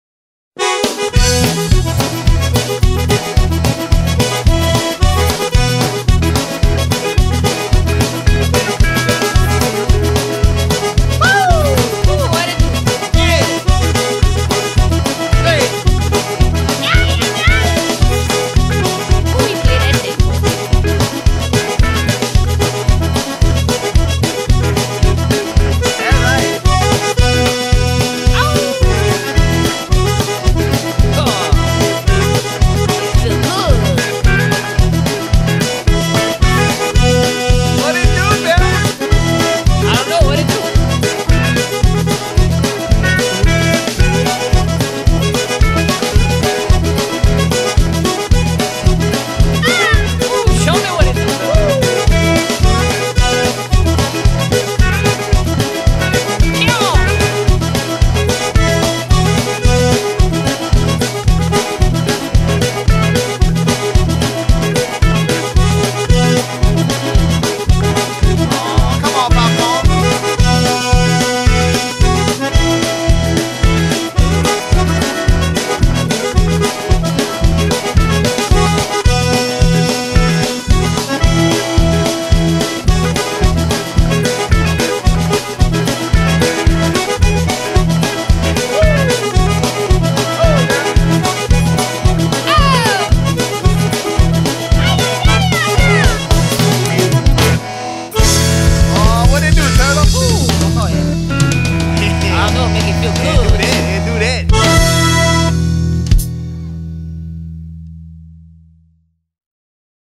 Cajun Permit on the Fly
Catching sheepshead on the fly in the Jackson Cuda